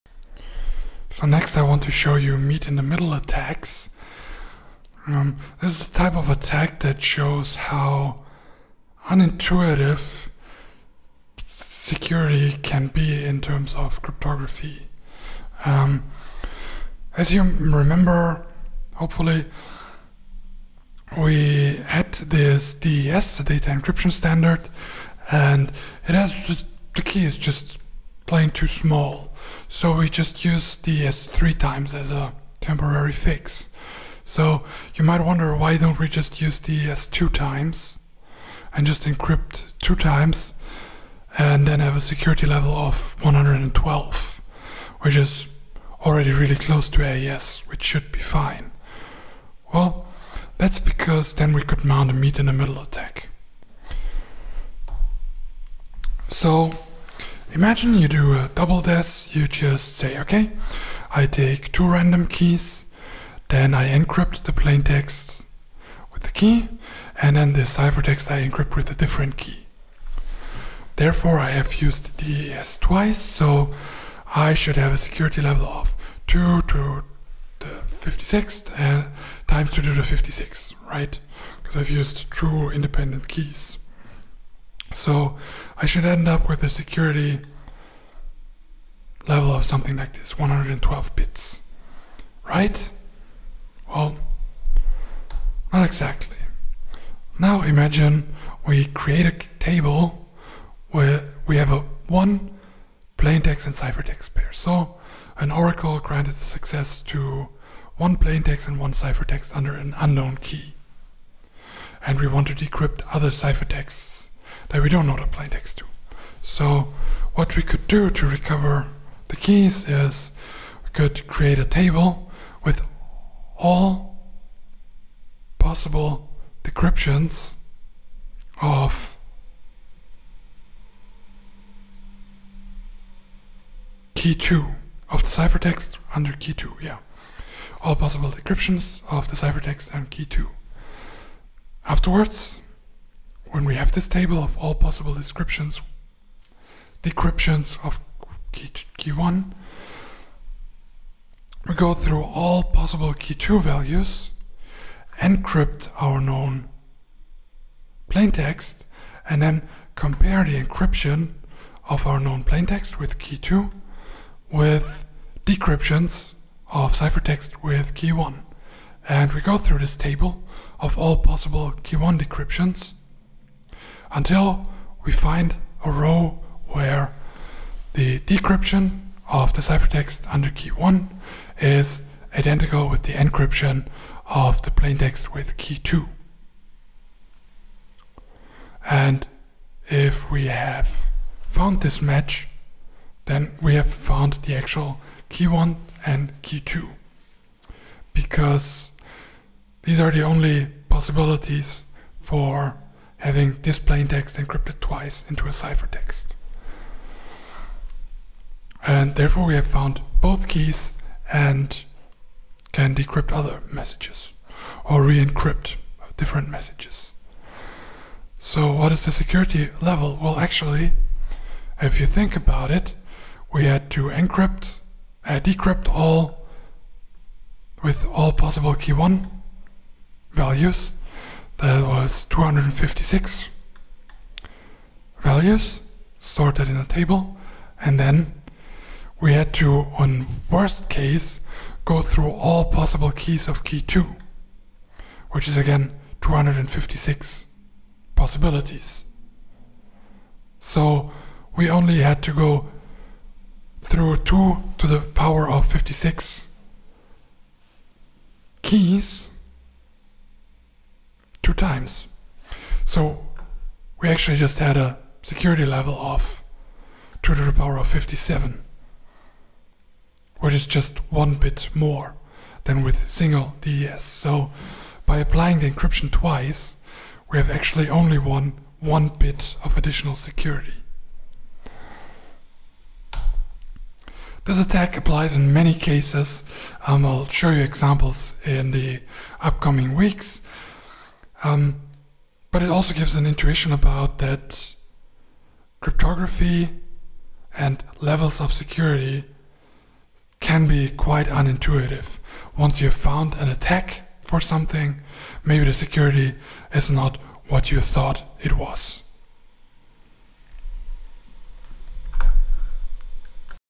03-01-03 Meet in the Middle Attack
This video shows how non-intuitive cryptography can be by the example of mitm attacks.